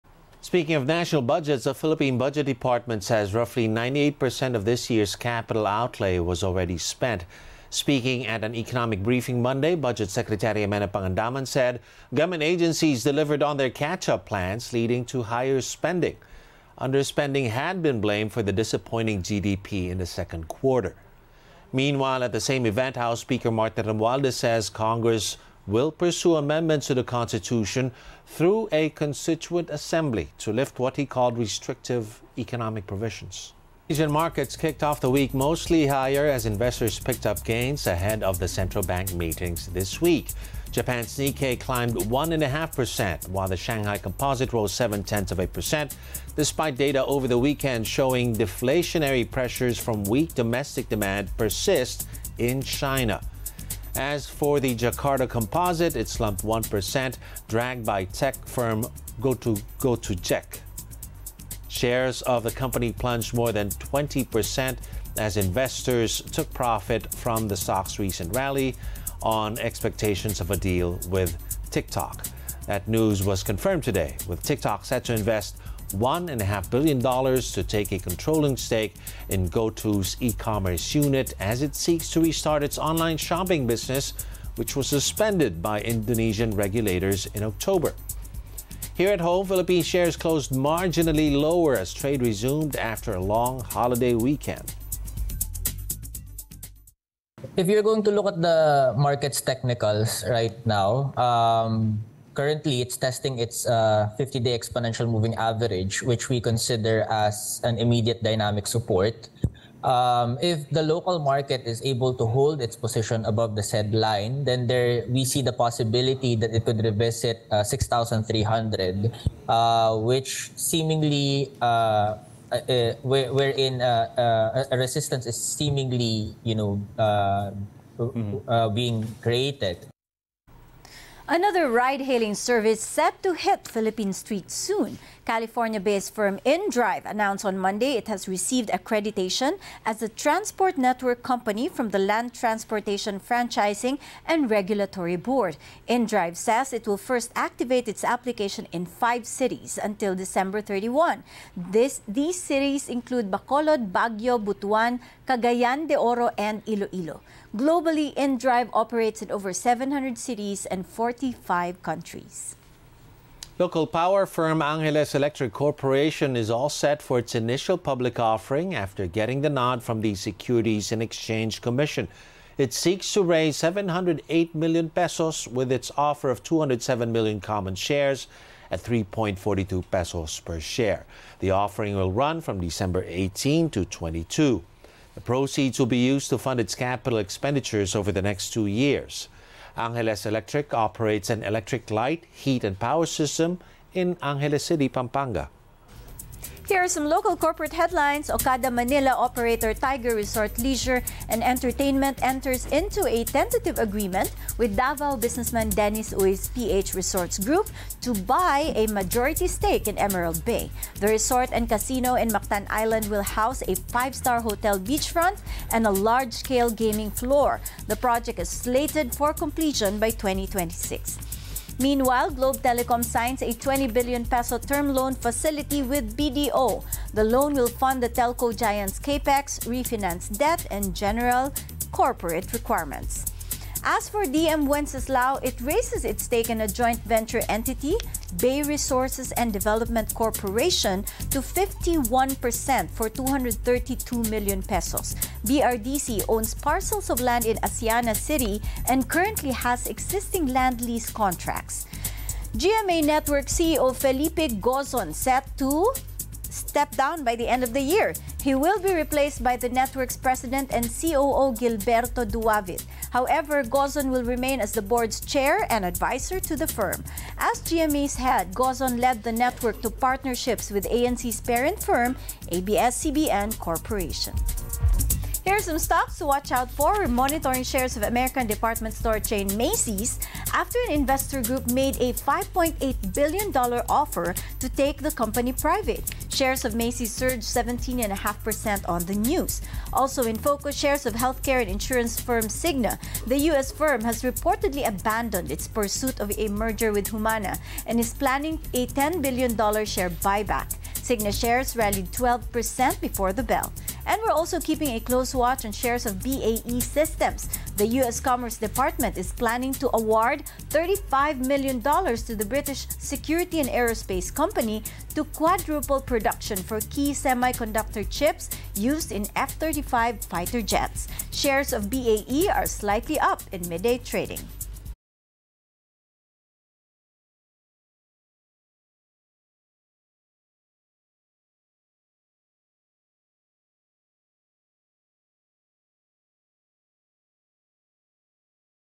DBM: Gov't spent 98% of over P5.2-T 2023 budget | ANC
Business Outlook: The Philippine Budget Department says roughly 98% of this year's capital outlay was already spent.